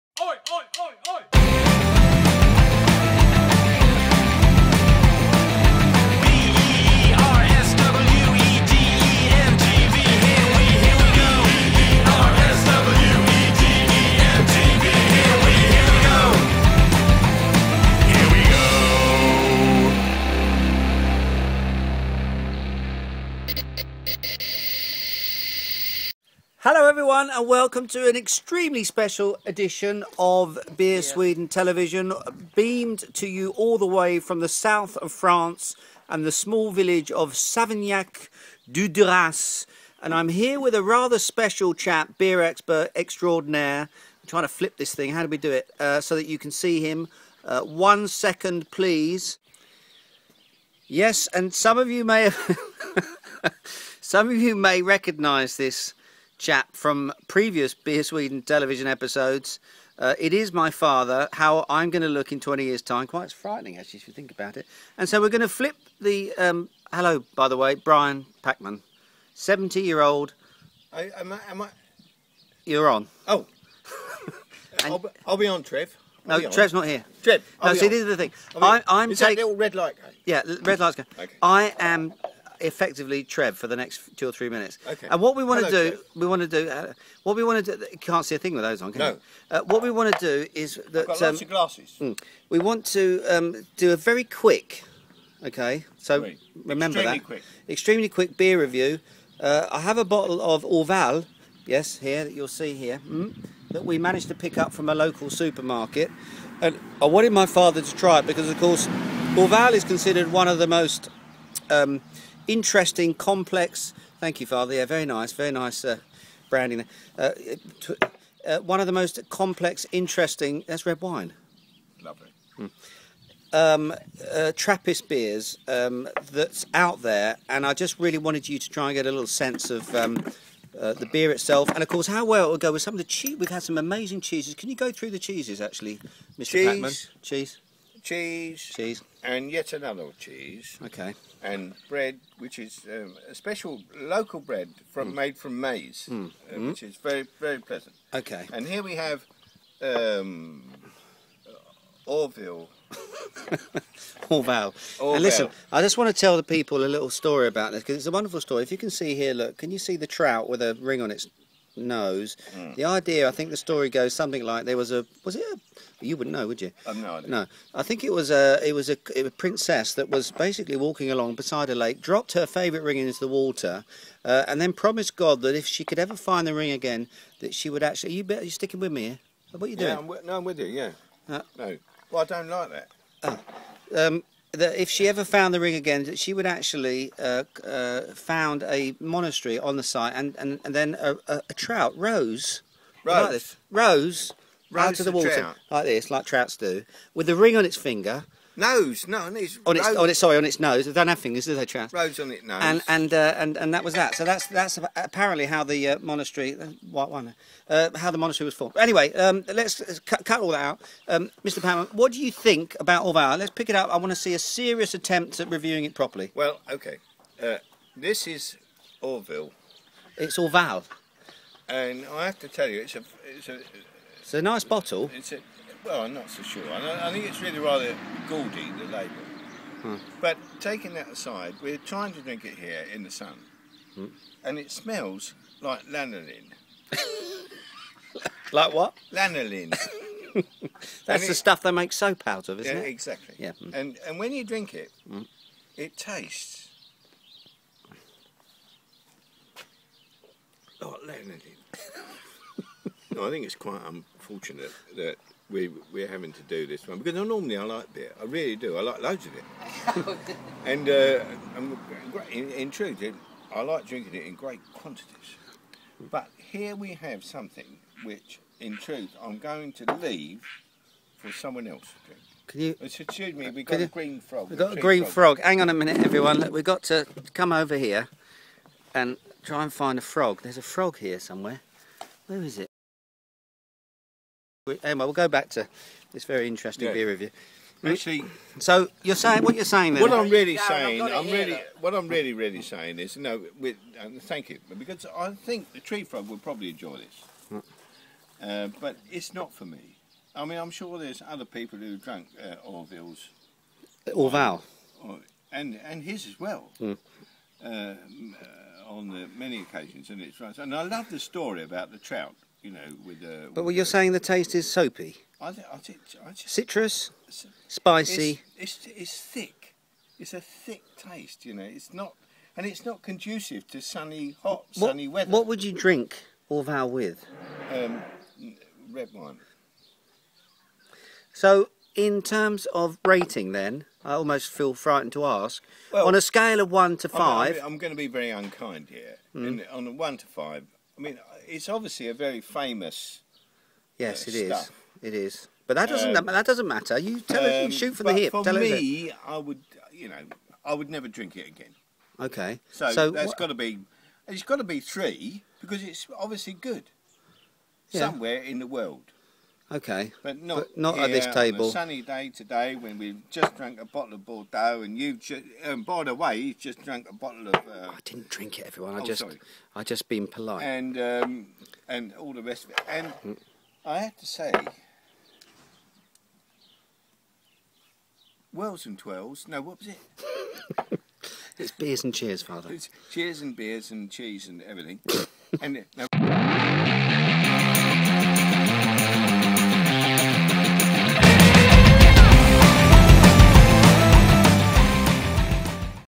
BSTV EP132 - Orval (the lanolin review)
It probably wasn't the best idea, trying to film a beer review in deepest France with my wine-loving father on his 70th birthday. Especially when he didn't have that many clothes on. But he insisted, and it was his birthday after all.
Sorry to all the monks at Orval. Or should that be Orvil.